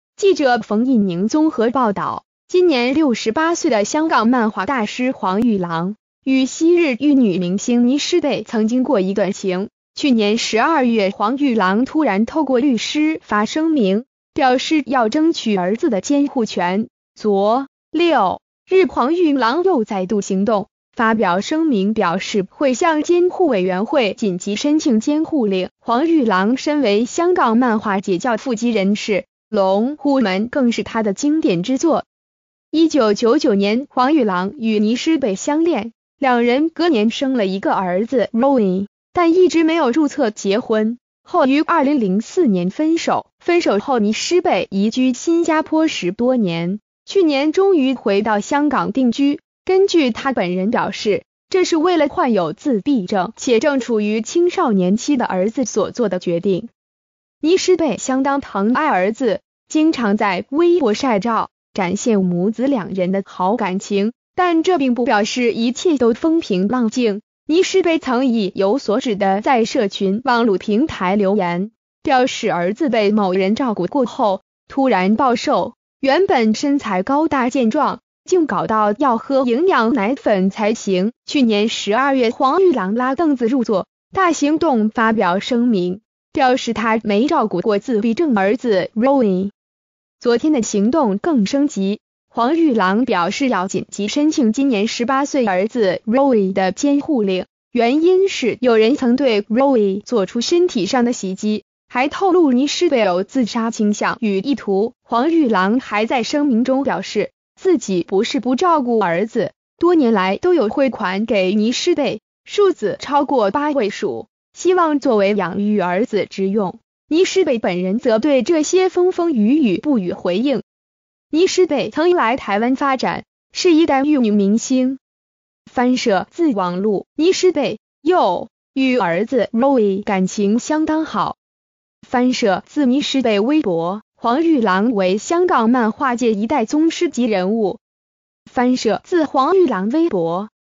香港漫畫大師急發聲明 控自閉兒遭襲擊
〔記者馮亦寧／綜合報導〕今年68歲的香港漫畫大師黃玉郎與昔日玉女明星倪詩蓓曾經過一段情，去年12月黃玉郎突然透過律師發聲明，表示要爭取兒子的監護權，昨（6）日黃玉郎又再度行動，發表聲明表示會向監護委員會緊急申請監護令。

發表評論評論如下。